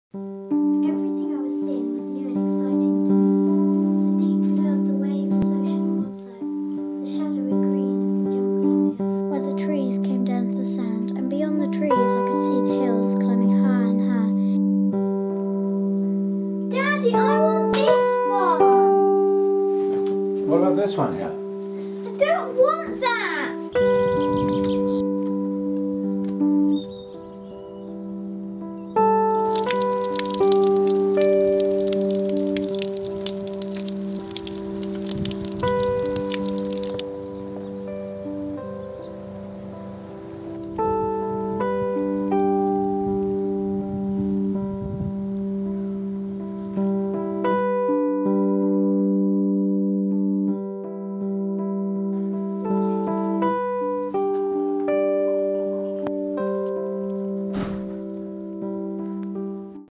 The Teddy Bear - Short Film *Award Winner*
This is a short film I made for a short film competition. The subject was emotion and the film had to be 1 minute long. I made this short film which centers around a teddy bear, who experiences love and loss at the hands of young girl who neglects it, but finds love in an unlikely source.